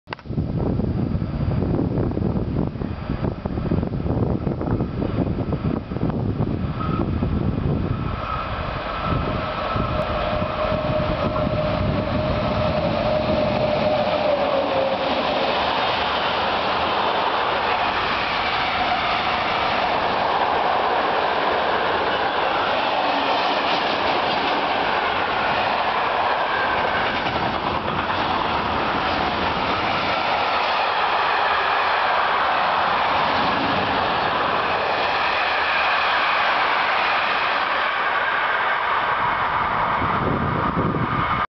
Bhf.Königslutter Gueterzug Doppeltraktion (8)
Bhf.Königslutter  (27.Juni 2013)
Gueterzug Doppeltraktion, Richtung Magdeburg.